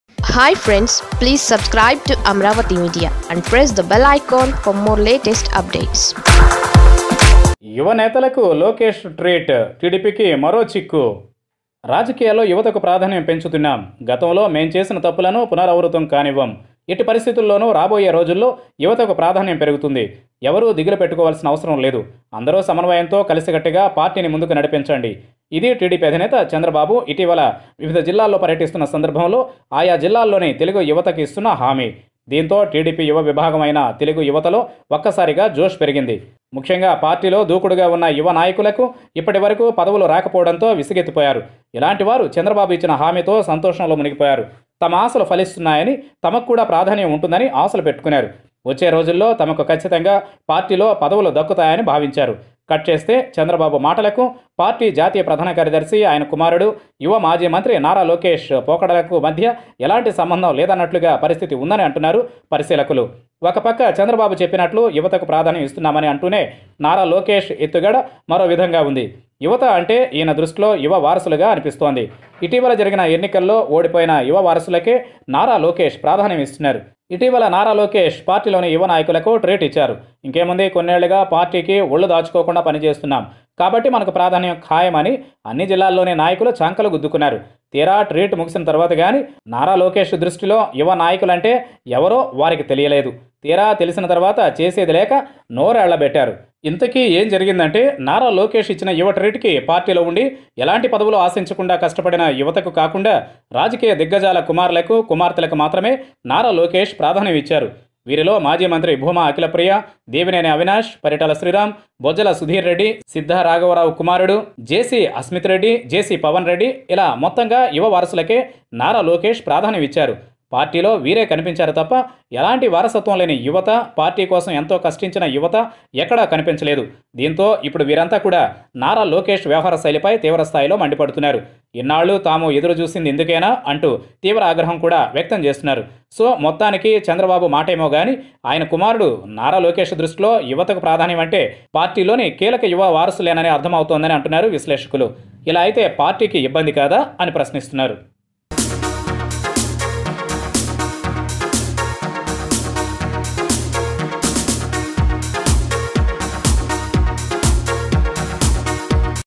టీడీపీకి మరో చిక్కు తెచ్చిన యువనేతలకు లోకేష్ ట్రీట్..!| Lokesh Creates another headache to the TDP.!
The TDP Chief Chandrababu Naidu is making efforts to make youth a priority in the Party.
But his son Nara Lokesh is acting contrary for that.